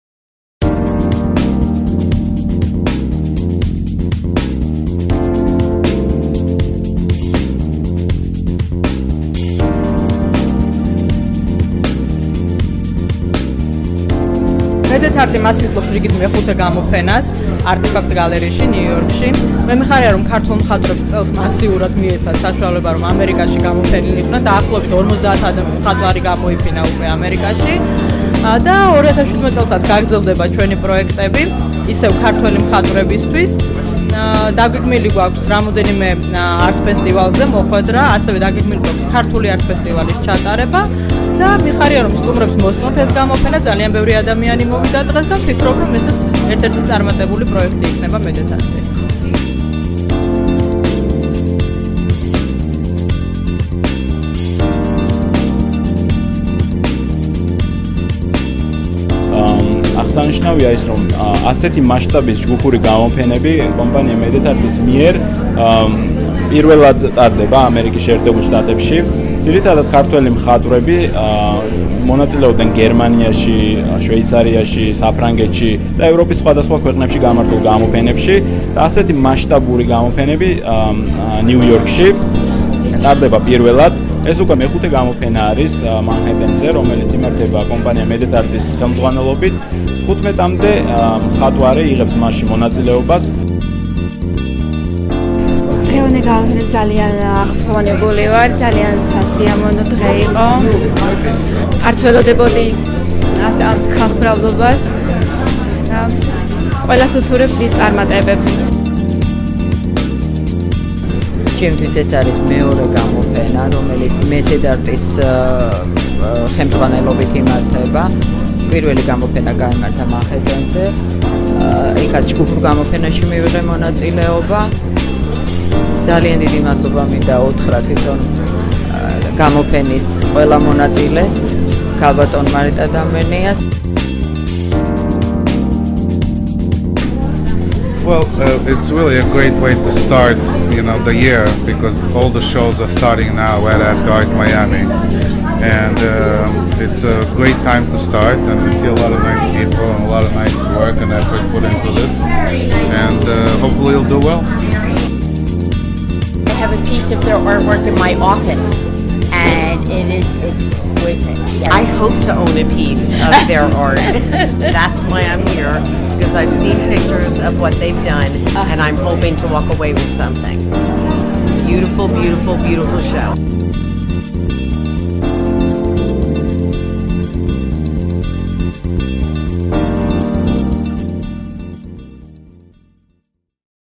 Madatart Georgian Artists Group Exhibition at Artifact Gallery
Madatart held Georgian artists Group exhibition at Artifact Gallery. 17 contemporary Georgian artists participated in the exhibition with more than 50 works of art.